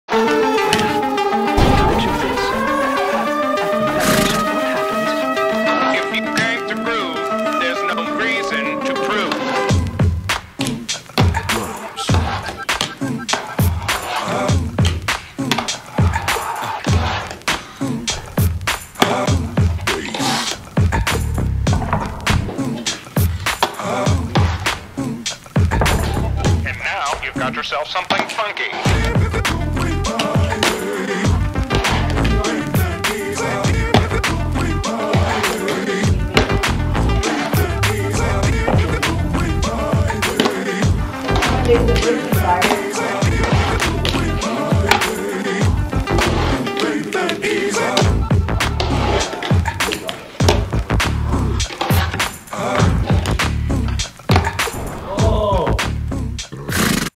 Fingerboard part short
Random fingerboard tricks. Just having fun on my fingerboard park. Another hippo instagram short.